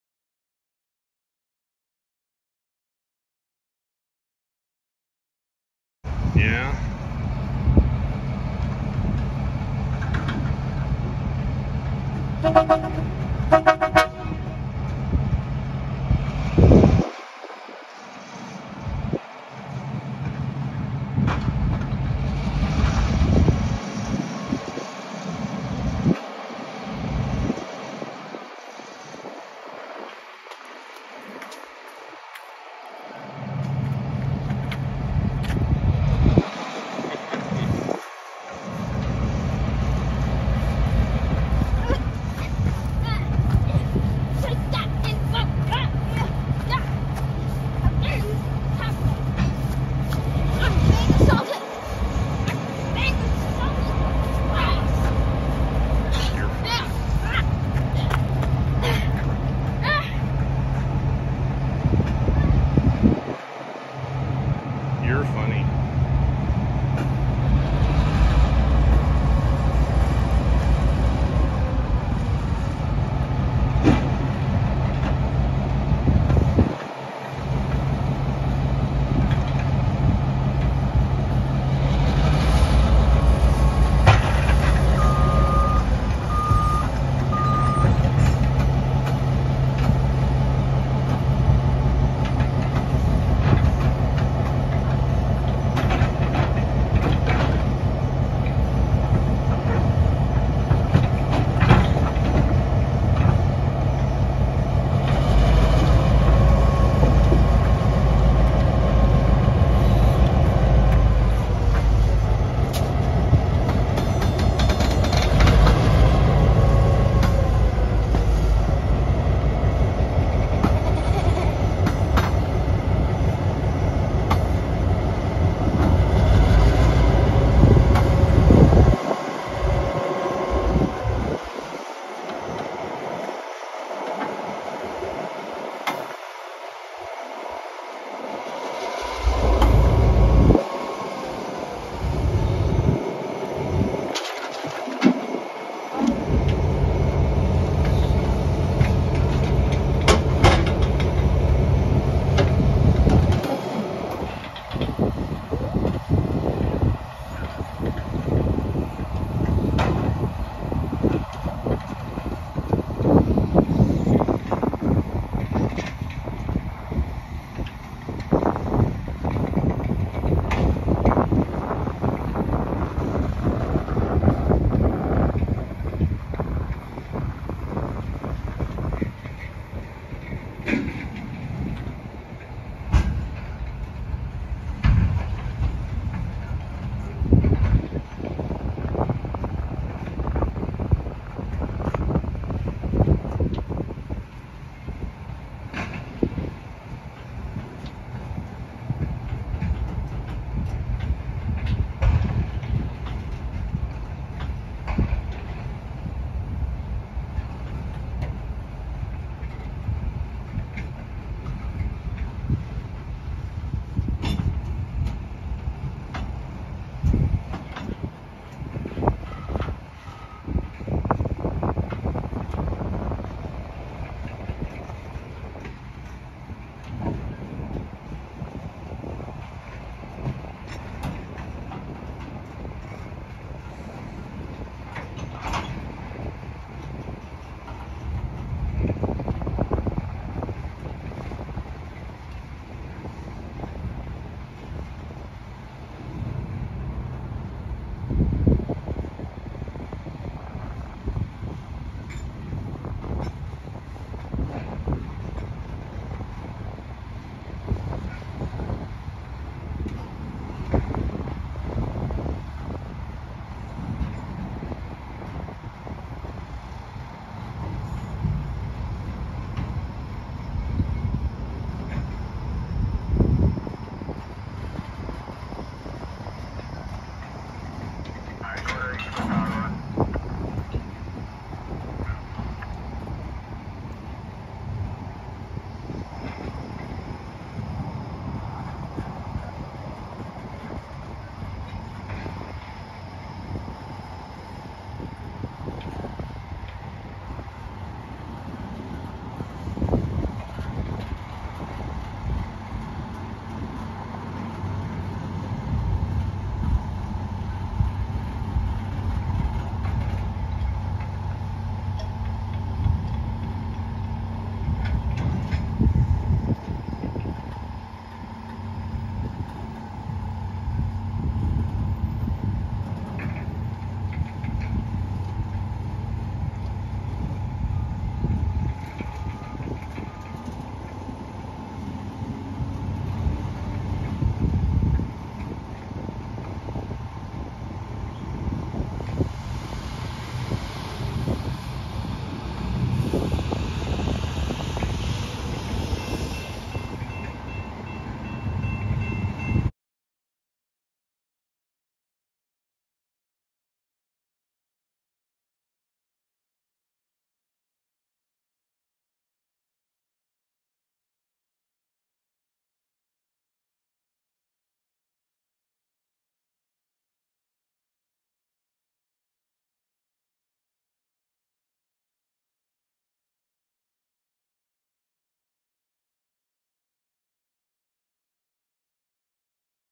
Tie Train on the NS
I finally got to film a GREX locomotive leading a MOW train in Slidell, Louisiana on Saturday, November 11,2023. It’s laying out ties on a Norfolk Southern siding.